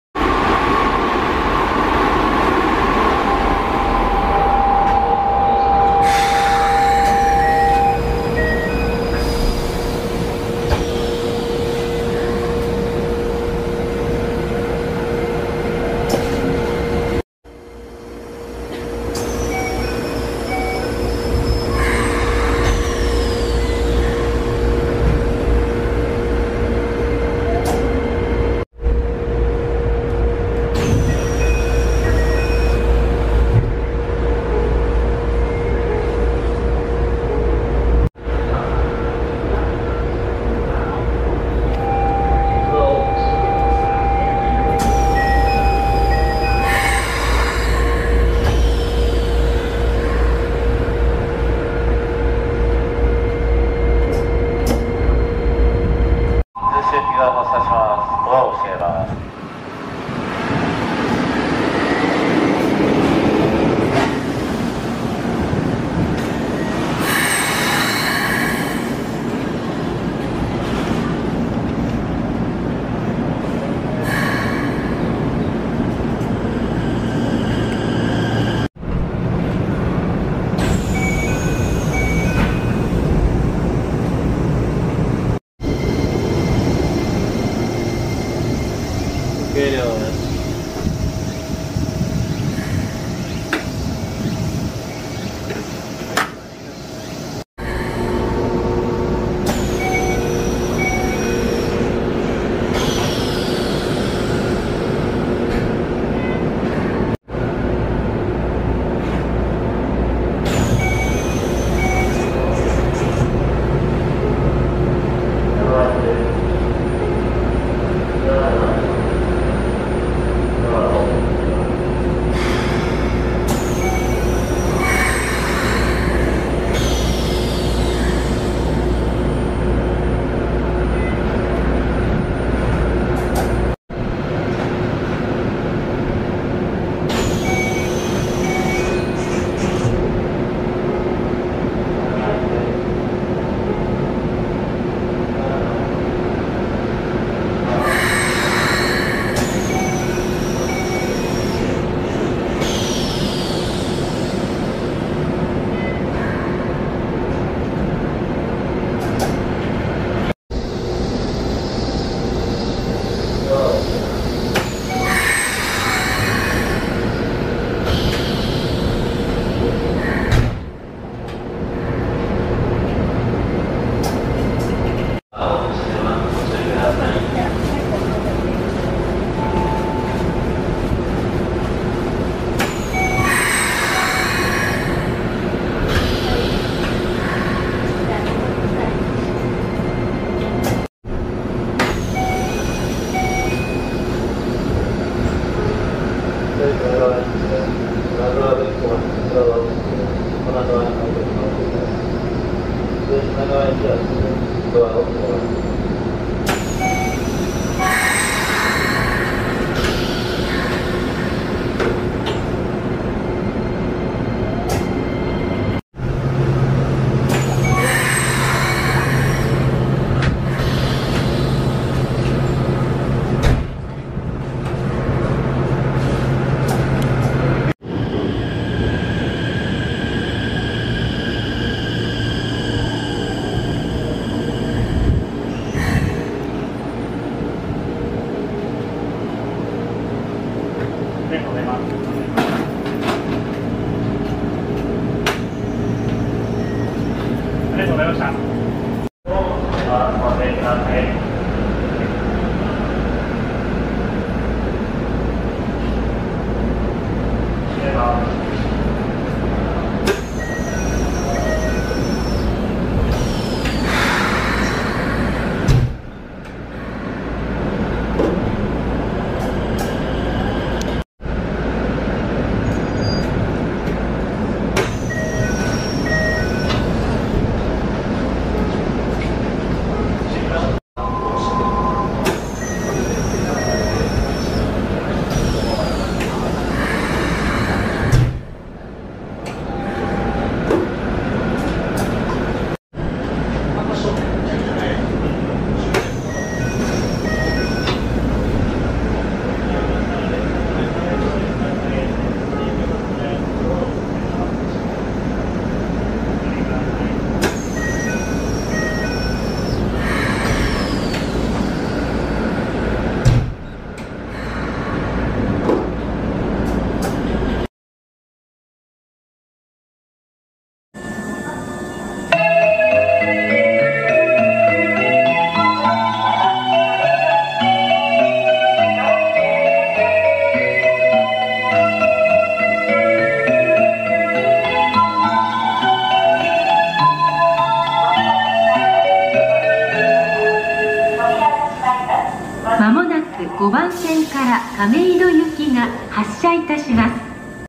【1177編成以降!!】京急新1000形 最近増備された編成のドア開閉シーン集
ドア開閉シーン再生リスト

1177編成以降の6両編成・8両編成のドア開閉シーン集です。
編成によって、開閉速度が異なります。

●撮影機材
Canon PowerShot SX720
●編集アプリ
KINEMASTER